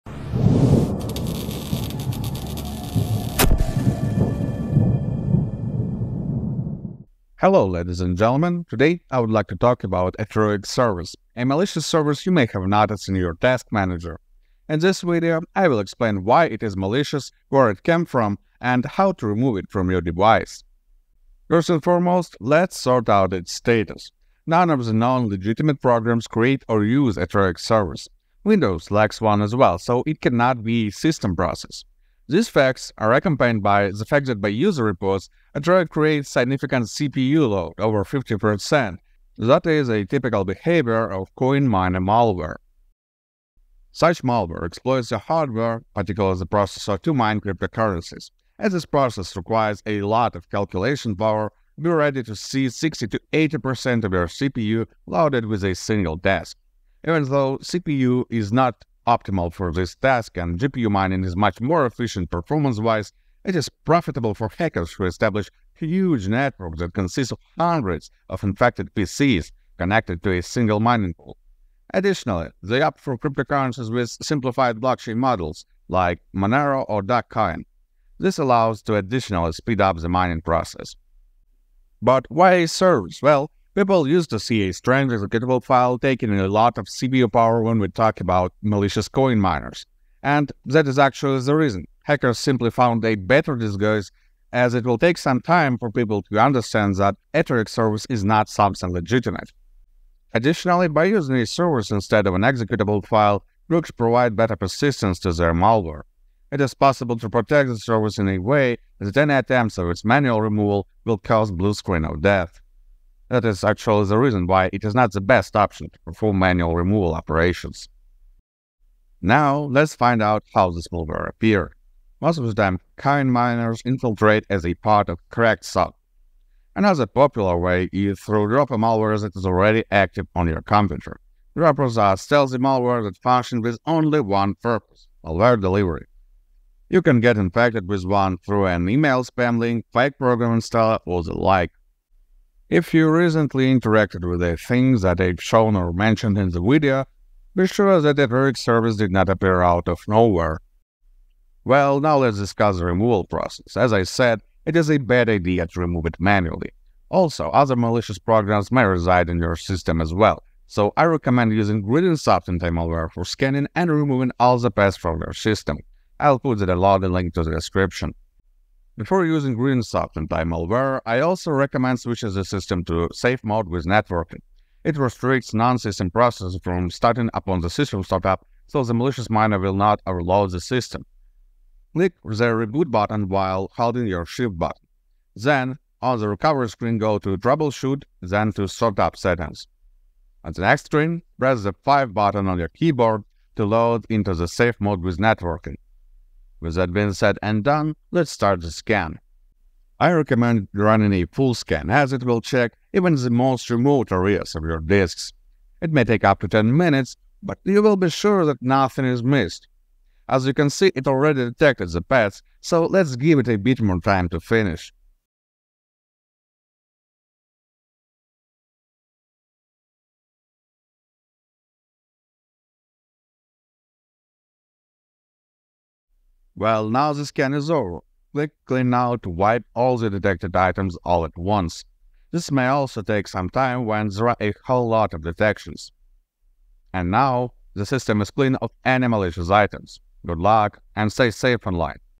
What is Atruic Service and How to Remove It?
Atruic Service is a strange service you can notice in the Task Manager. This service is most likely associated with coin miner malware active in the system. Among usual symptoms of such malware are high CPU load, that comes particularly from Atruic Service, and the presence of strange processes in the startup. How to remove Atruic Service from the PC? And is it legitimate? We answered these questions in the video.